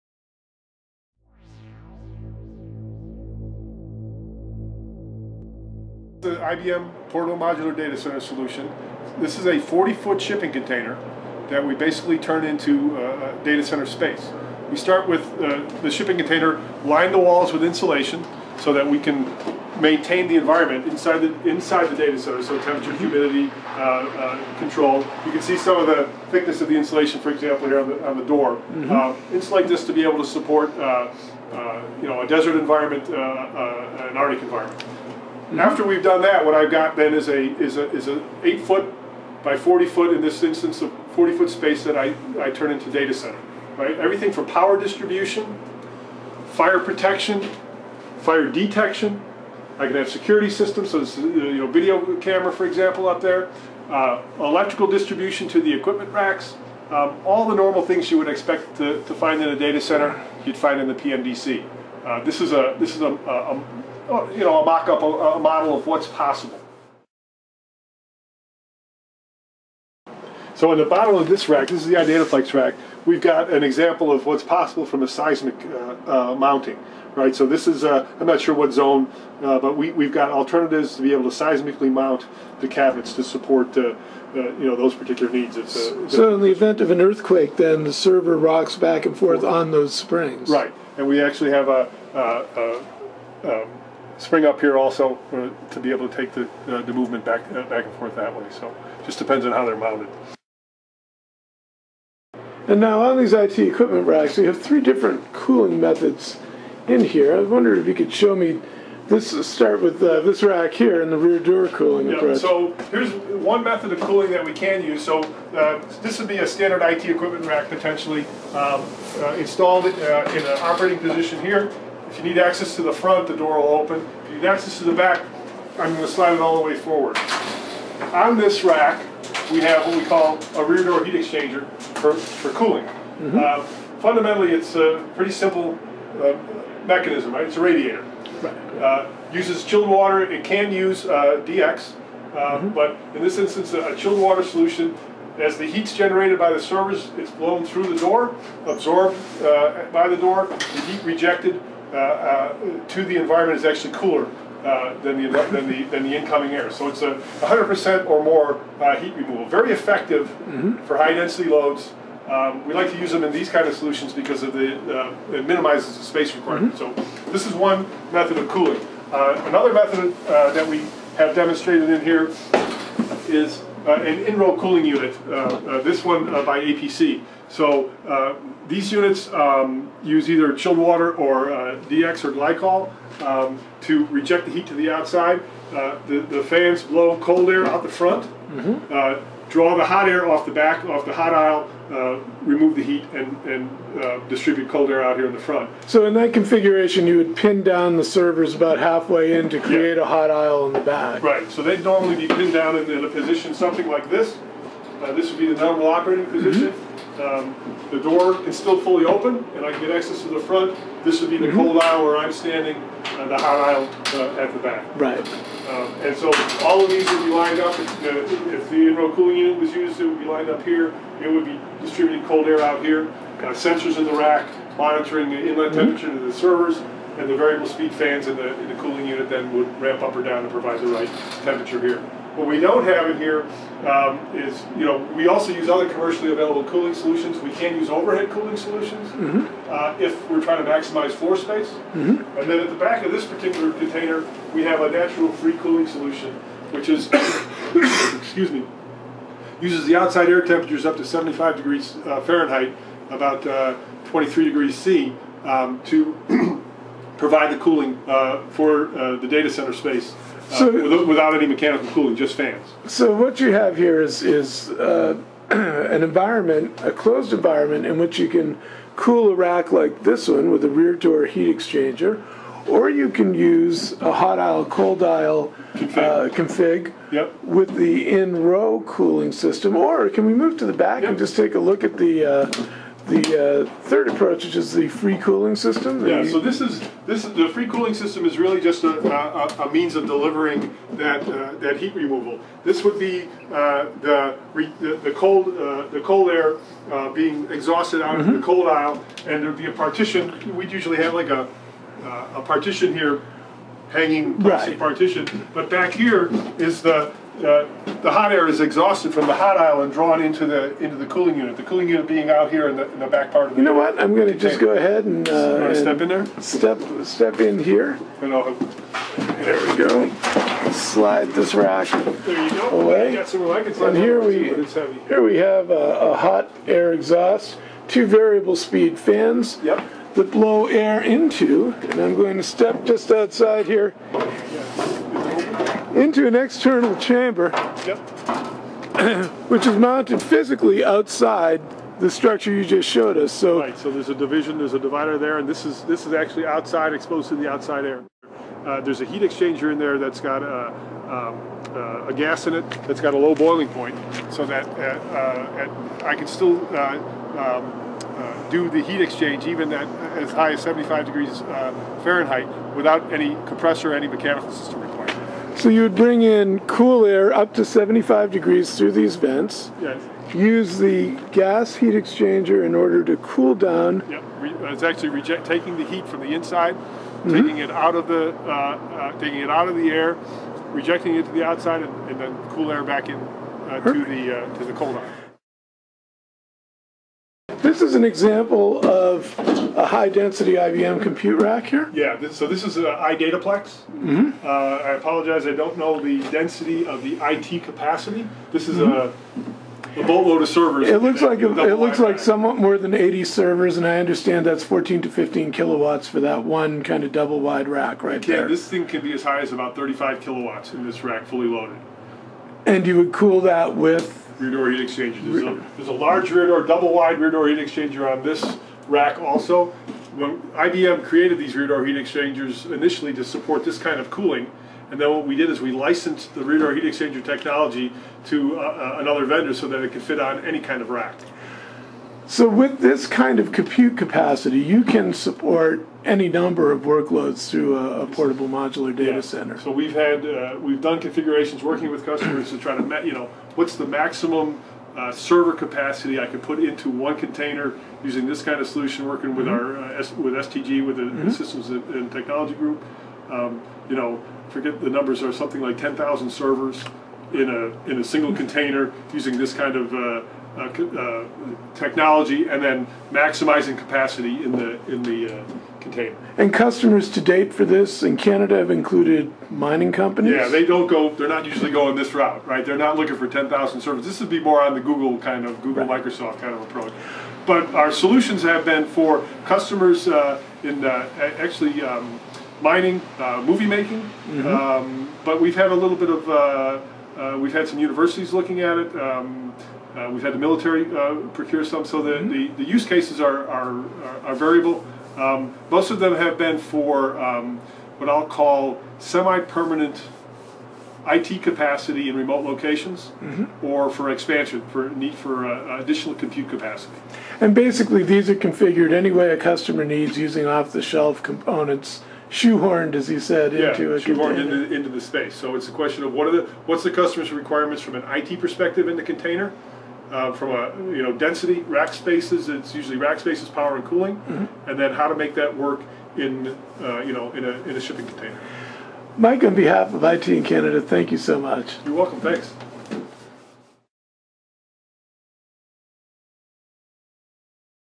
IBM Portable Modular Data Centre.wmv
Michael Hogan guides us on a tour through IBM's Portable Modular Data Centre when it makes a stop in Calgary